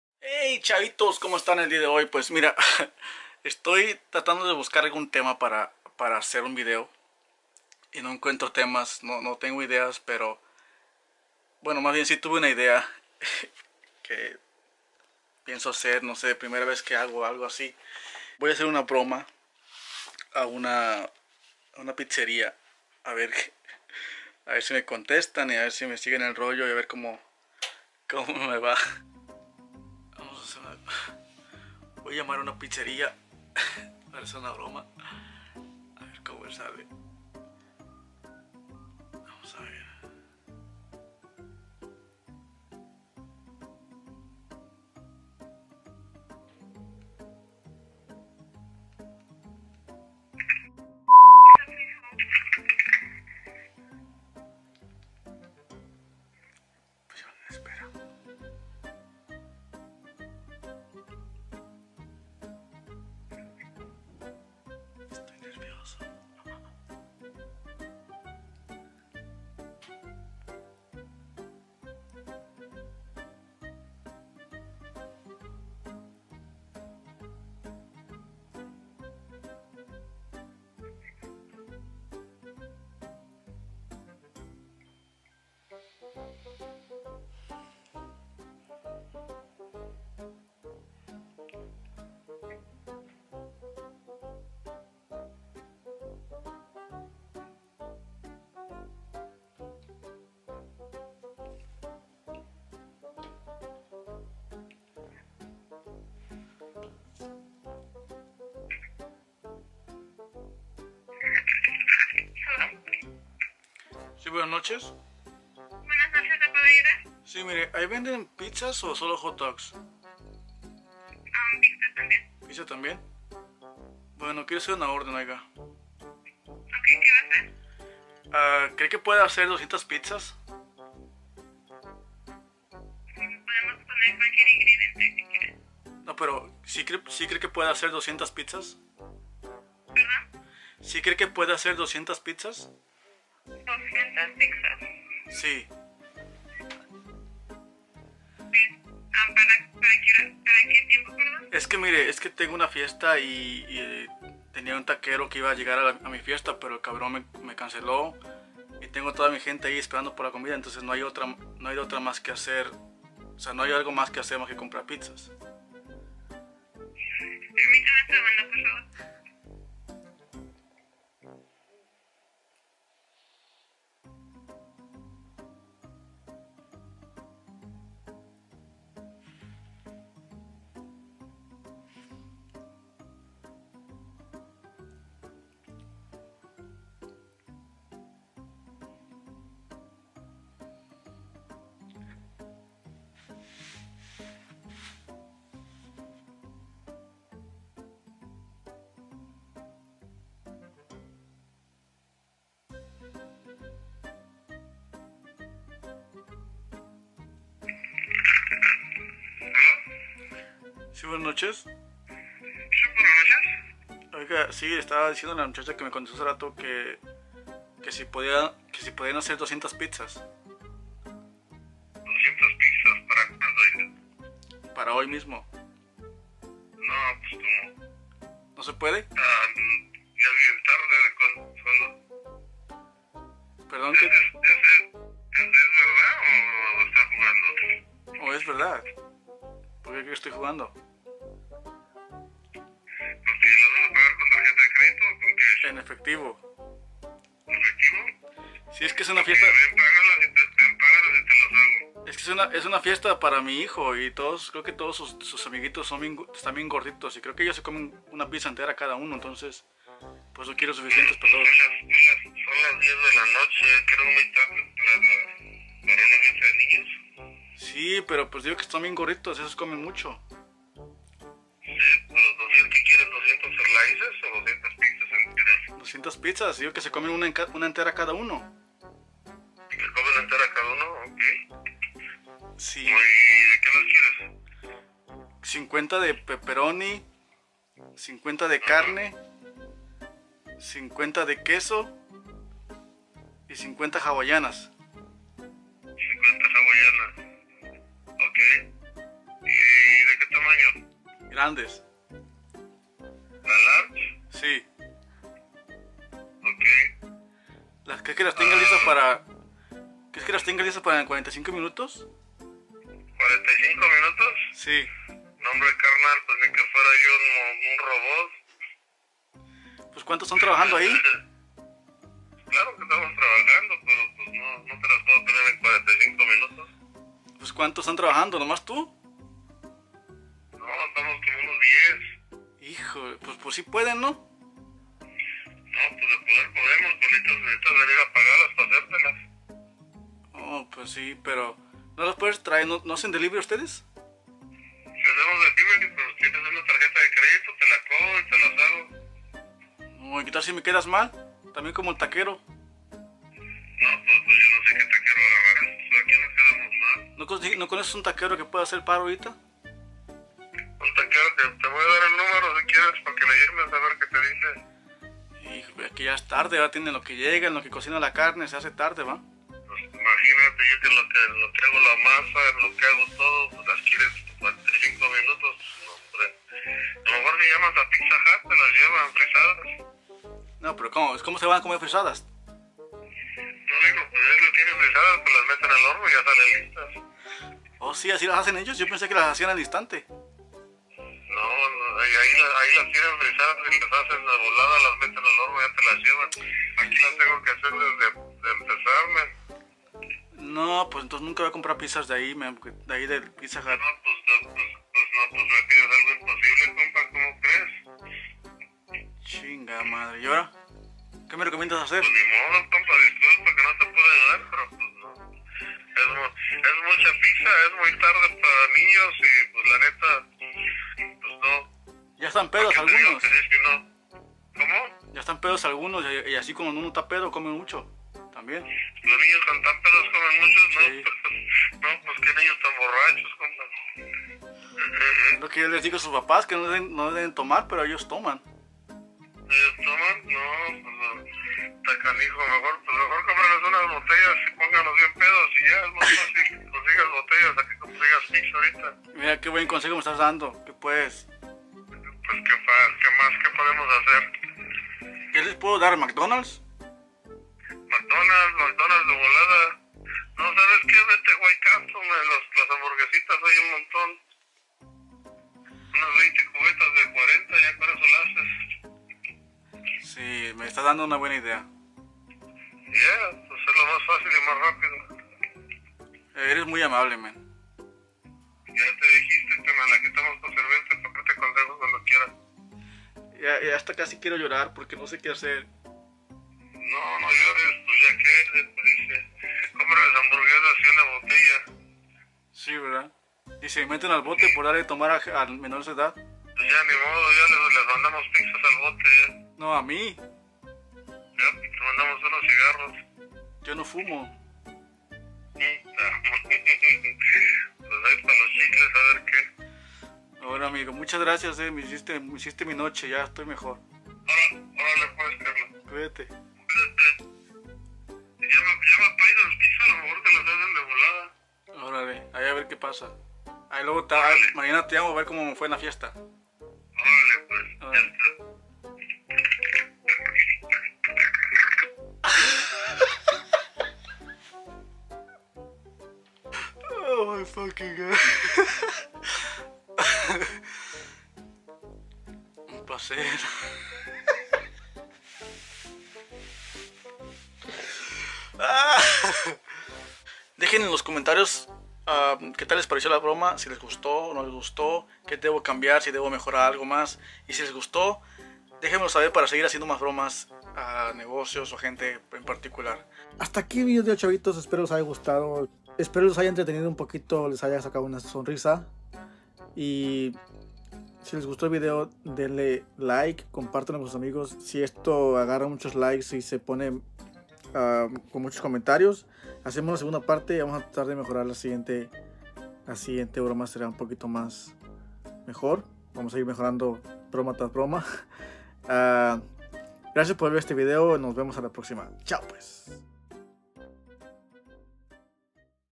Ordene 200 pizzas | Broma a una pizzería | Omar Avila
Hice una broma a una pizzería

Primera vez haciendo bromas 🤣

---¿DONDE ME PUEDES ENCONTRAR?